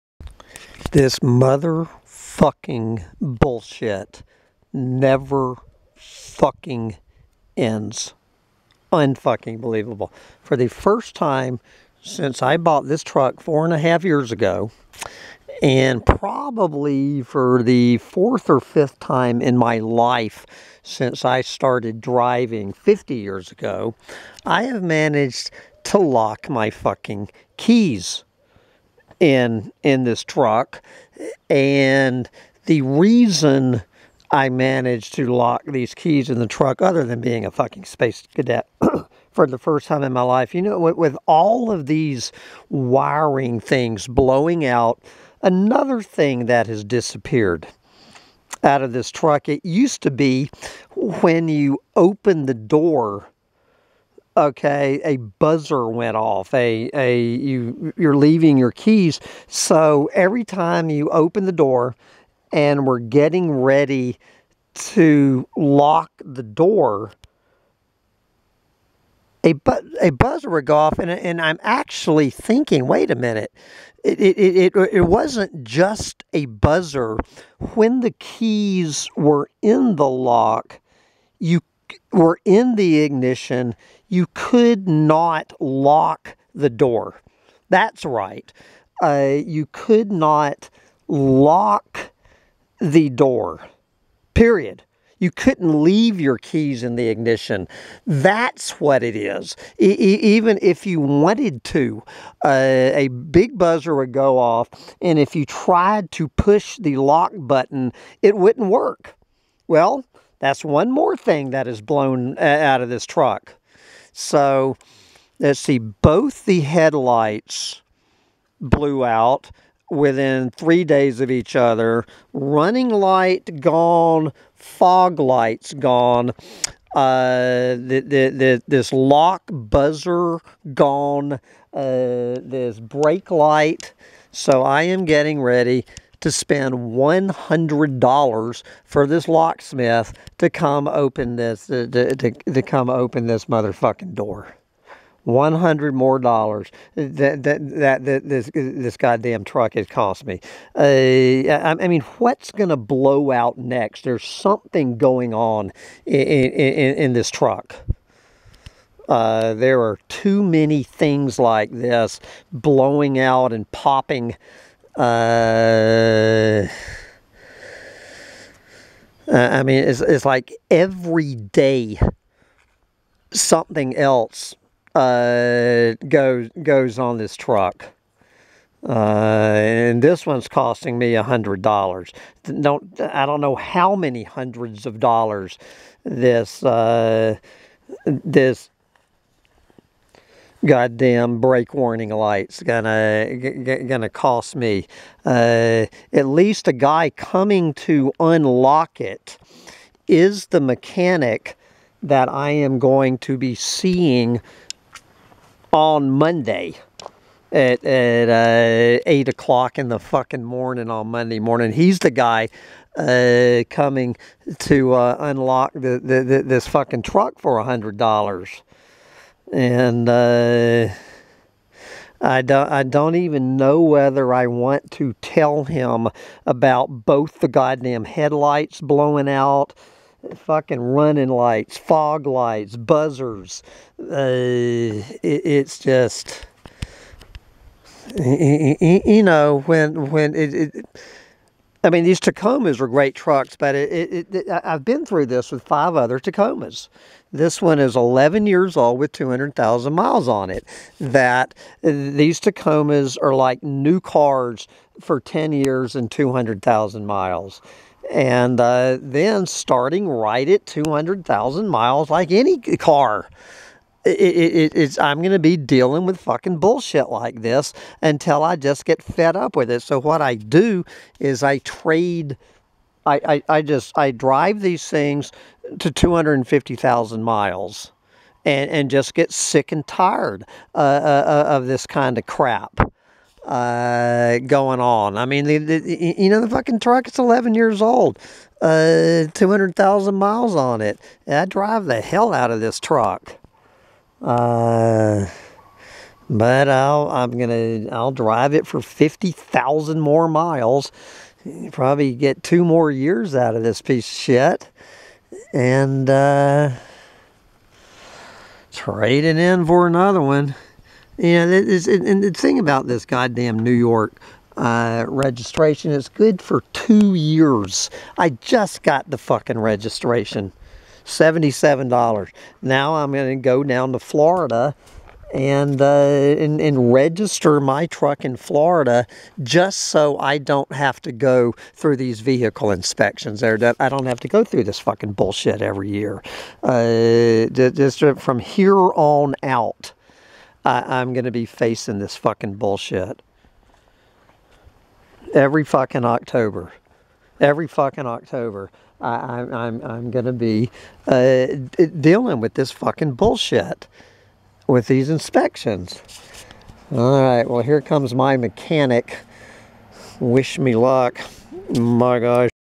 A Pathetic Fossil Fuel Slave Throws Another Hundred Bucks Down the Toilet
I shudder to ask if anyone knows whether there is a fuse that controls the accidental lock protection, and if so, what the Hell is it called?